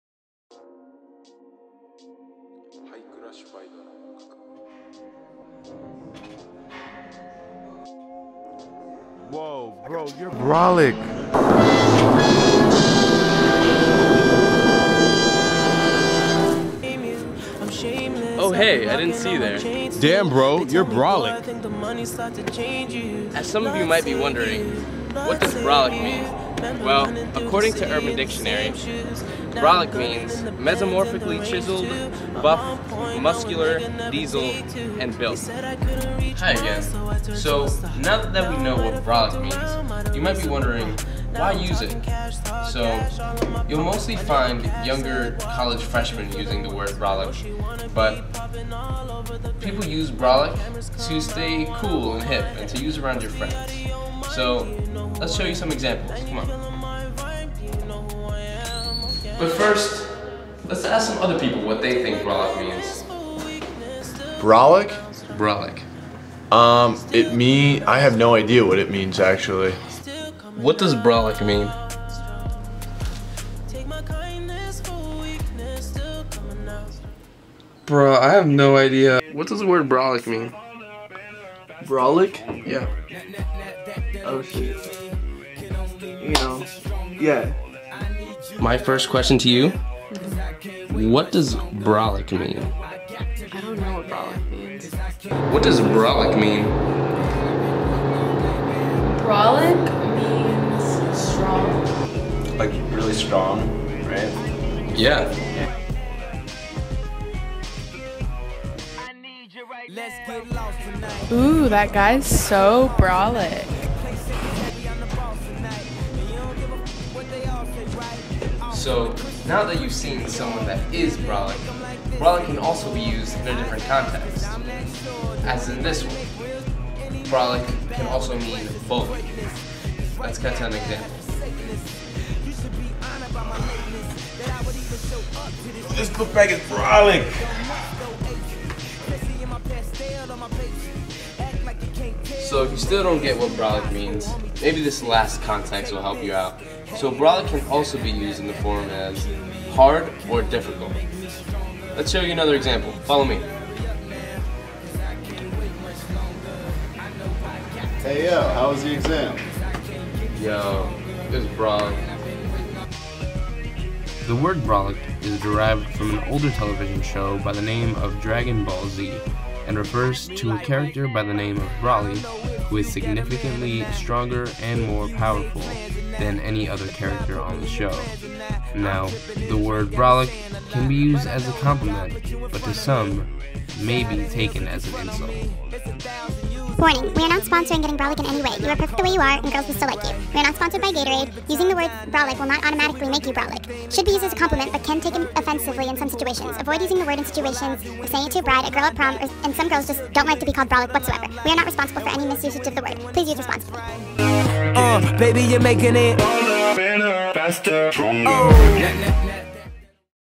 So What Does Brolic Mean?
A short film on the definition of "brolic" highlights how the word is used and the many contexts it can be used.